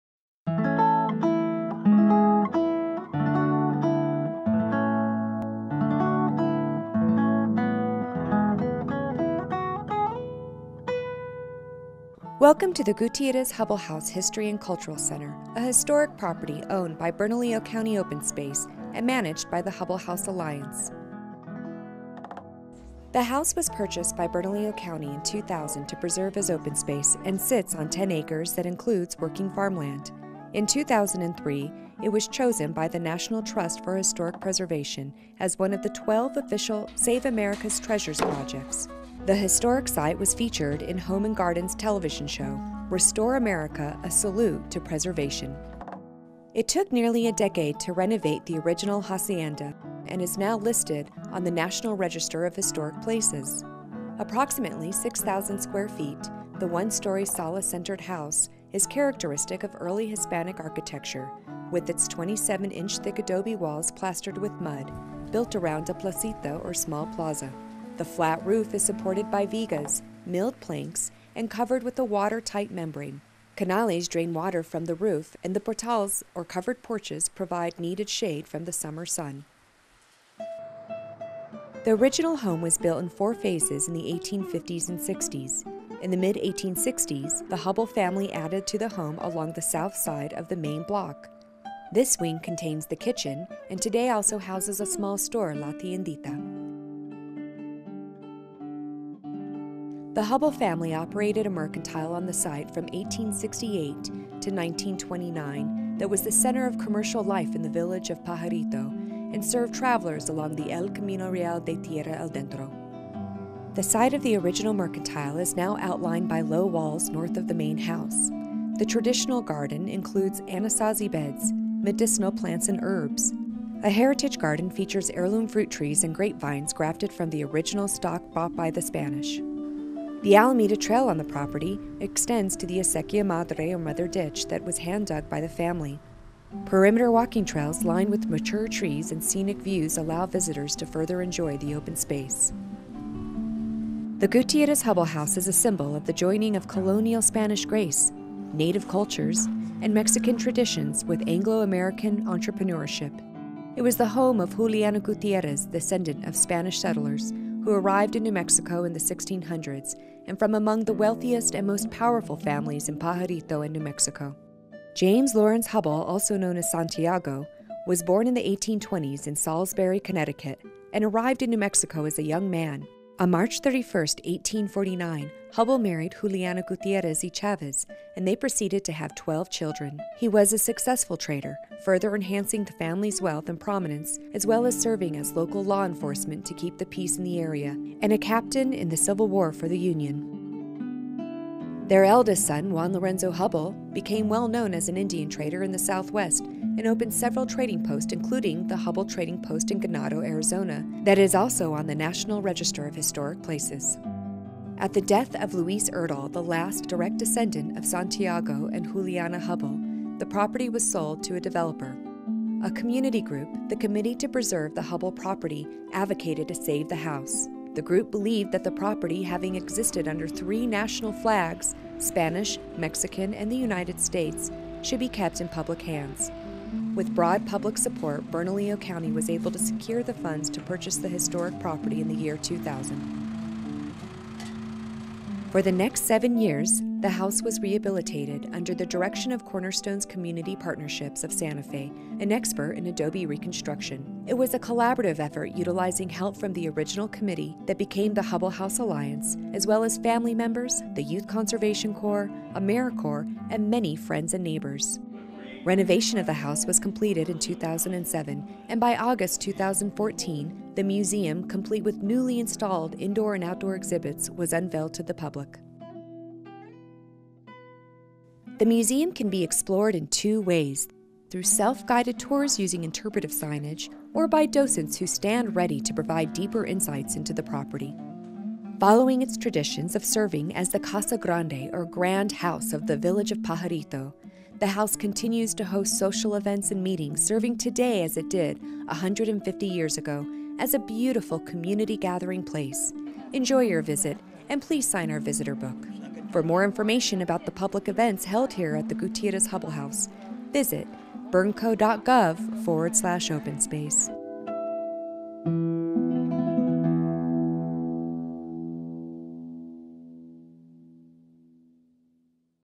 History of the Gutierrez Hubbell House in Bernalillo County's South Valley
File under history and culture.  Once a trading post on the Historic Camino Real, The Gutierrez Hubbell House is restored to its former glory after falling into disrepair over the years.  It's now one of the crown jewels of the Bernalillo County Open Space Program and the South Valley.